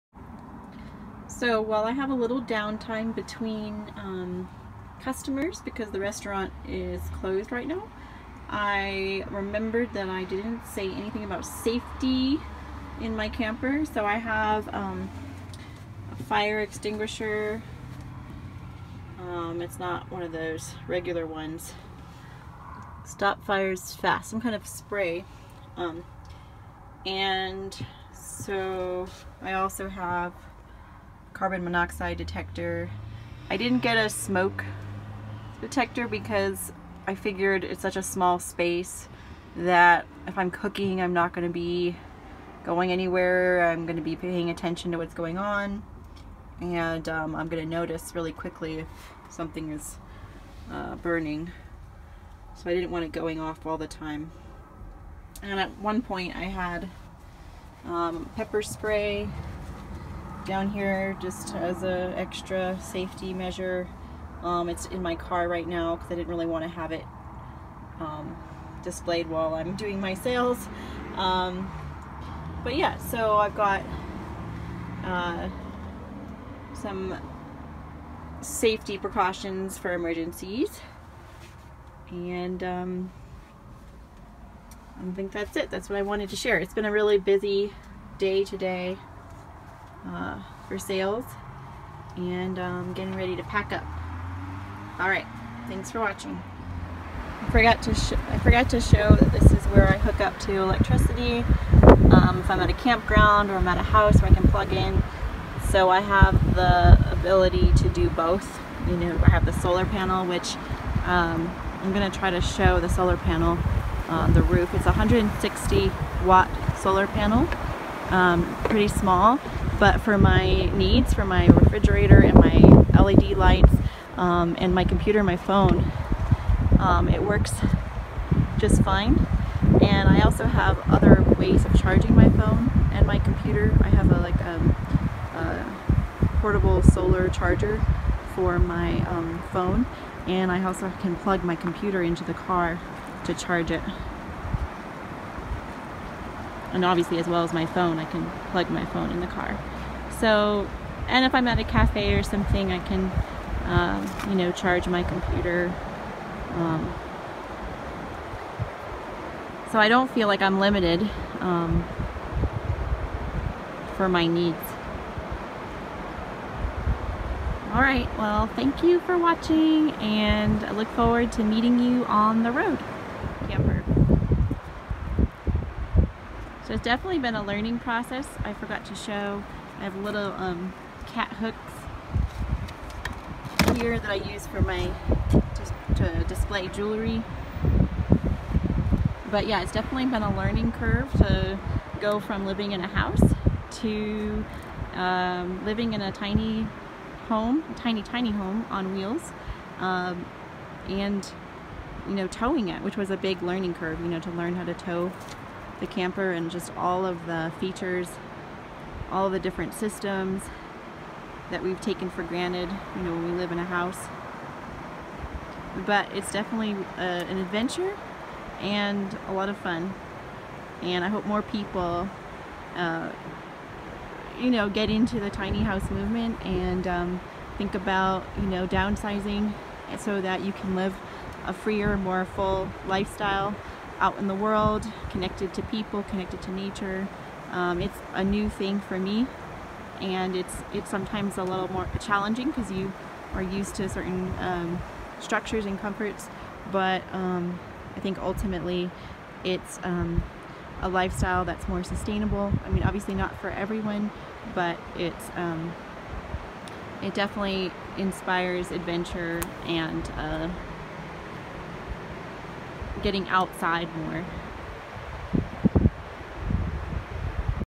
Woman Converts Camper to an ADORABLE Tiny Home on Wheels Video- Extra Features
Woman Converts Camper to an ADORABLE self sufficient tiny home on wheels/mobile boutique. I converted my micro bonair 1300 to a tiny home on wheels/mobile boutique! Here are some extra features. Please excuse the black bars!! ( Oops!) You can find me @wildcatsister on FB and Instagram. 😸

In this video I discuss safety features, solar panel, and the tiny home on wheels lifestyle.

Julia Eves Artwork